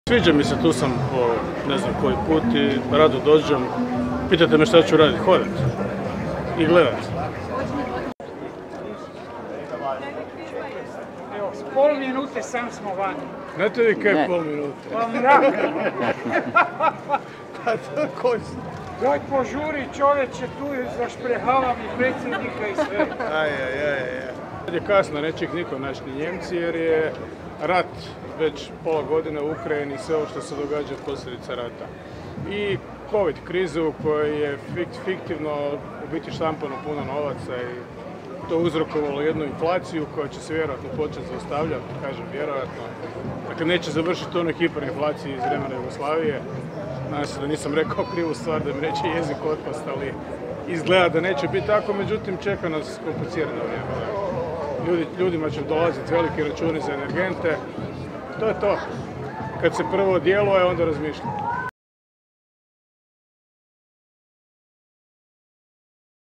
Milanović: Nadam se da nisam rekao krivu stvar da mi neće jezik otpast
Predsjednik Republike Hrvatske Zoran Milanović posjetio 24. Špancirfest i odgovarao na pitanja novinara.